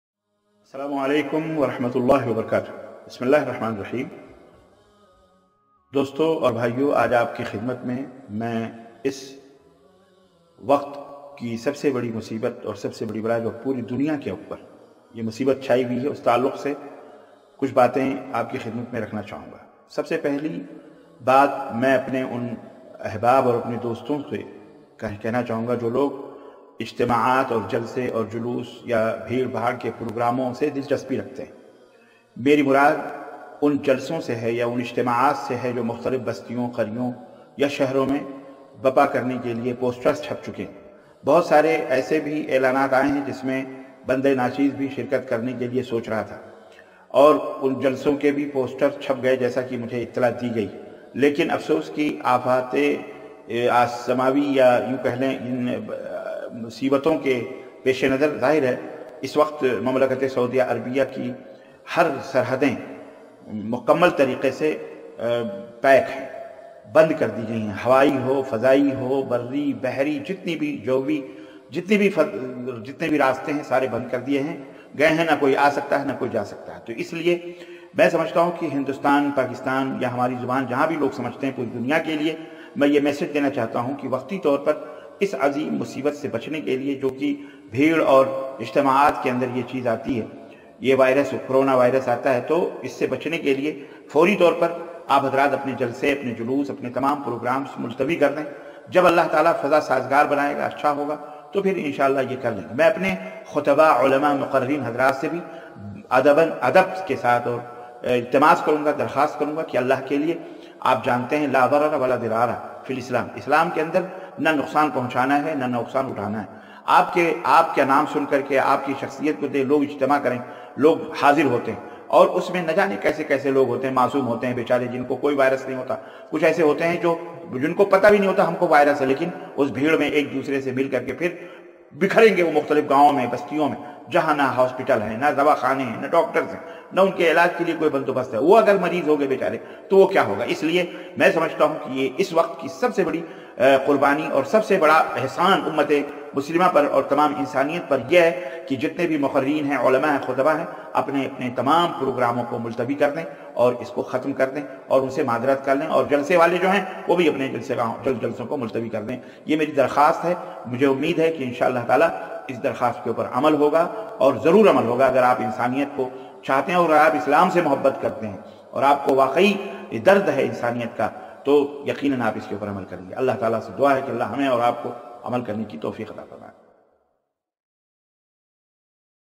Coronavirus | Ulama se Appeal | Dr. Syed Meraj Rabbani Madani
توجيهات لعلماء الهند باتخاذ الاجراءات الإحترازية بسبب انتشار فيروس كورونا للدكتور سيد معراج رباني المدني
Twitter.com/DrSMRabbani
Instagram.com/DrSMRabbani
Fb.me/MerajRabbani